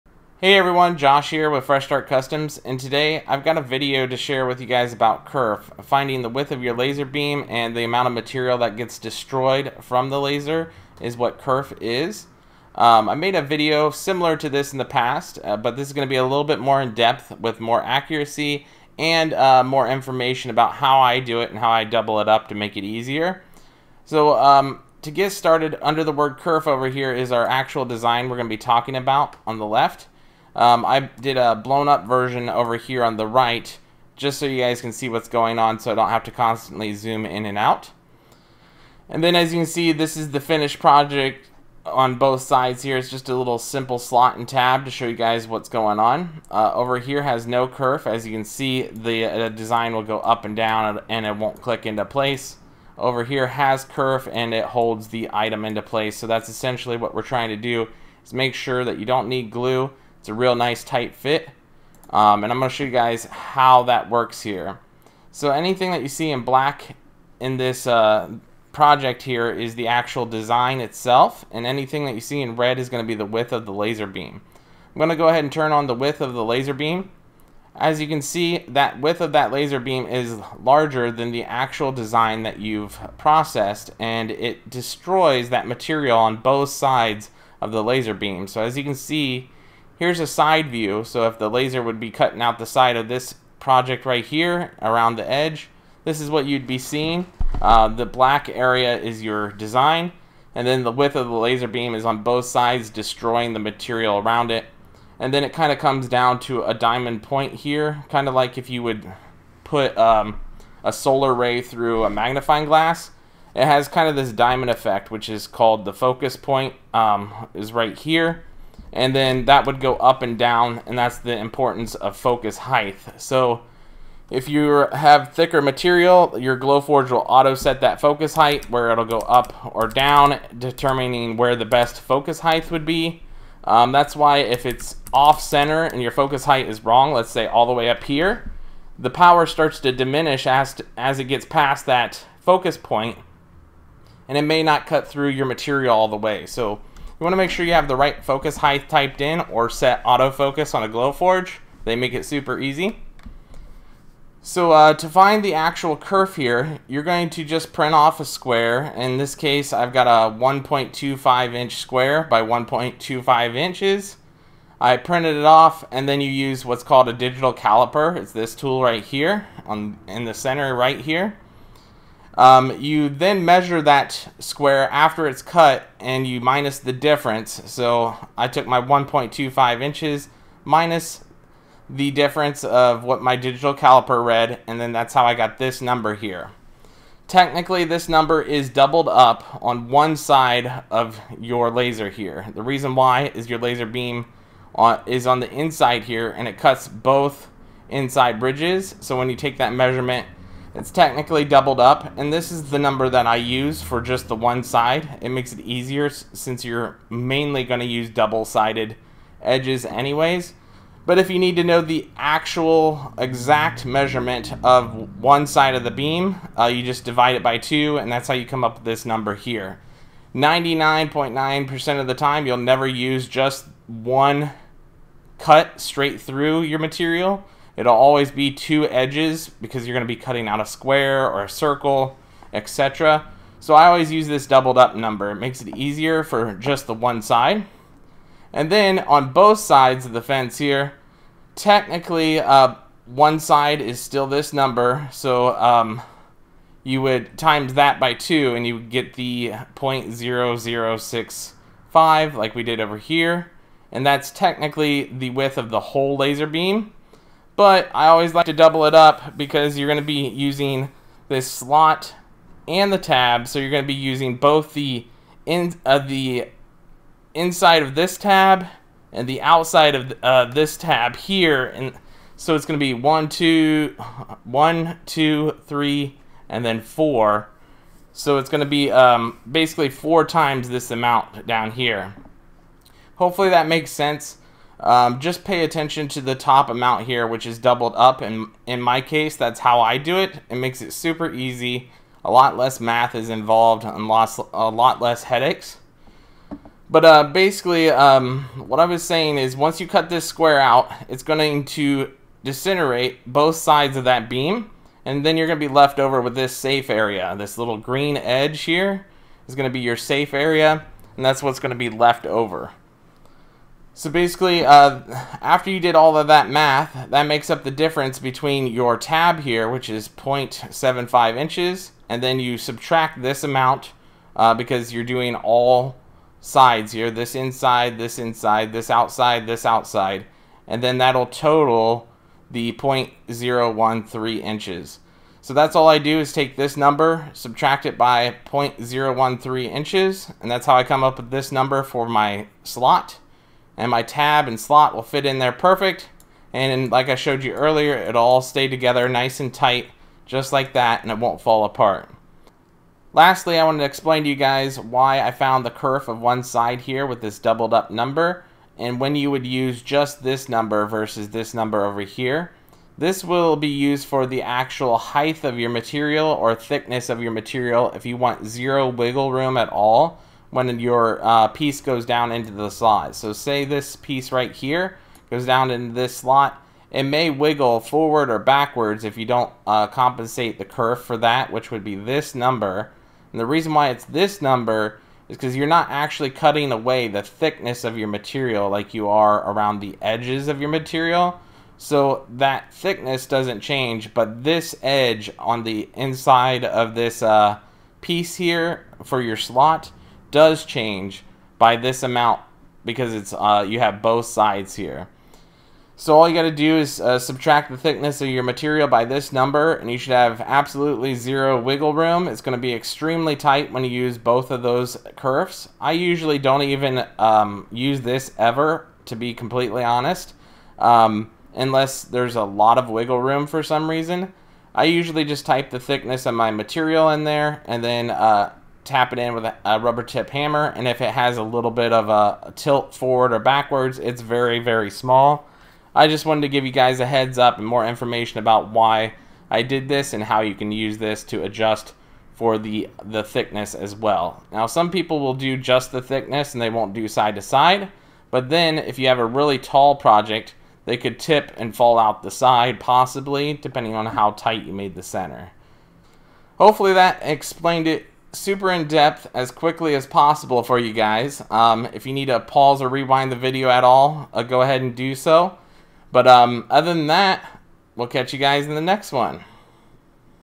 Understanding Kerf - Episode 6 - Glowforge Guide
Quick video showing how to find kerf of a laser.

then click on main list.